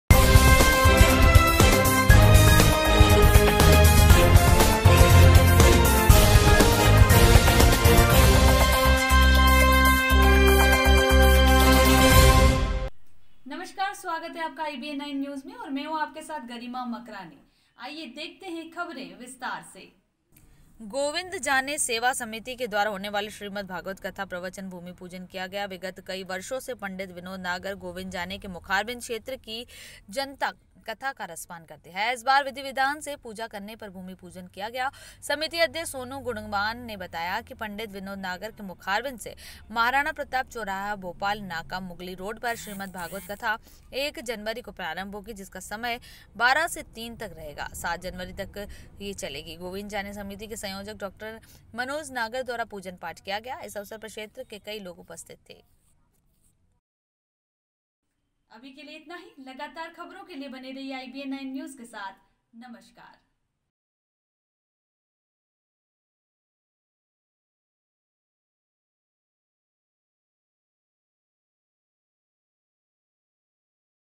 गोविंद जाने सेवा समिति के द्वारा होने वाले श्रीमद् भागवत कथा प्रवचन | IBN9
गोविंद जाने सेवा समिति के द्वारा होने वाले श्रीमद् भागवत कथा प्रवचन